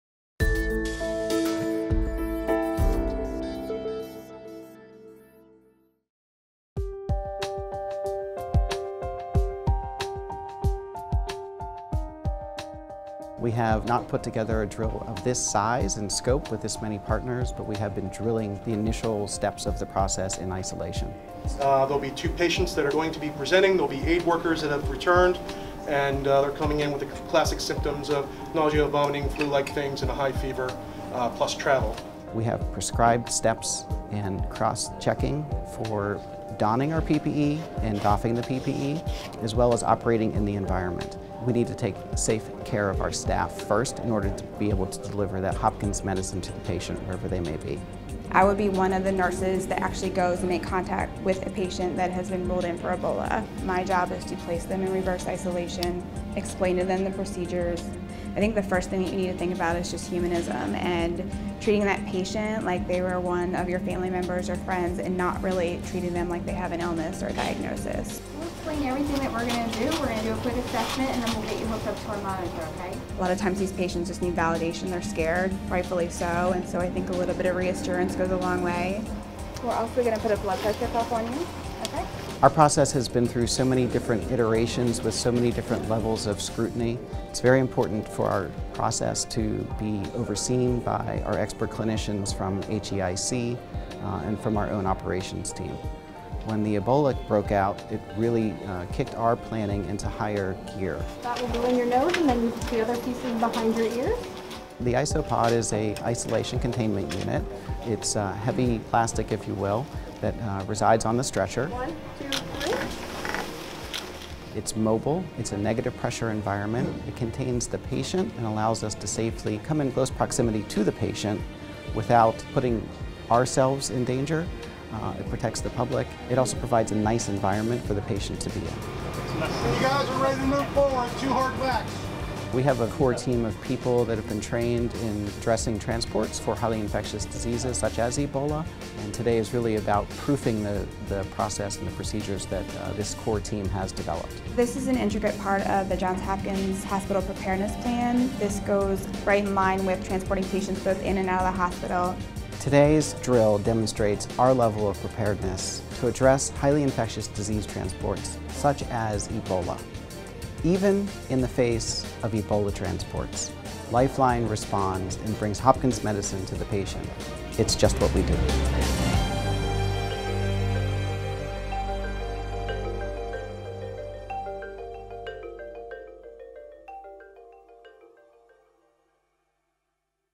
Ebola Patient Transport Drill | Johns Hopkins Medicine and Lifeline
Designated teams at all of the Johns Hopkins Medicine hospitals have trained for many months to respond to the likelihood that we would care for a patient with Ebola virus disease. To fortify these preparedness plans, a drill was conducted on Nov. 6 to simulate the transport of two patients with Ebola being transferred from Johns Hopkins Bayview Medical Center to The Johns Hopkins Hospital. As part of the drill, two people with symptoms of Ebola presented themselves in Johns Hopkins Bayview’s Emergency Department. Staff from Johns Hopkins Bayview and the Johns Hopkins Lifeline team donned and doffed their protective personal equipment, and Lifeline used specialized equipment to take them by ambulance to The Johns Hopkins Hospital’s Emergency Department.